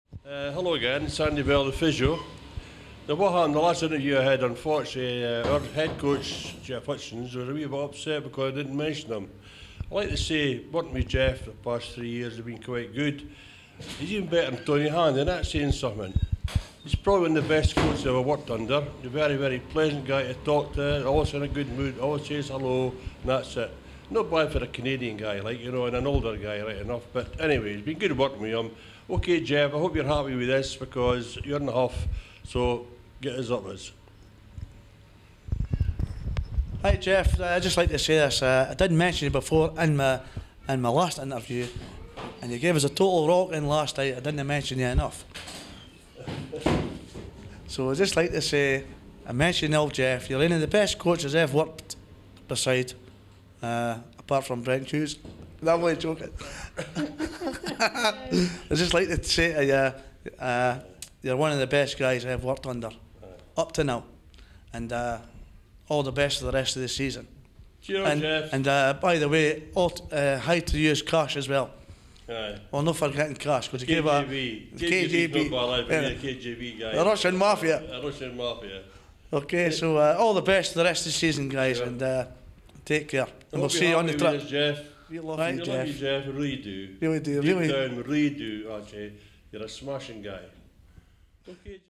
Kevin & Andy with a wee message for the coaches
Kevin Ward & Andy Bell with a wee message for the coaches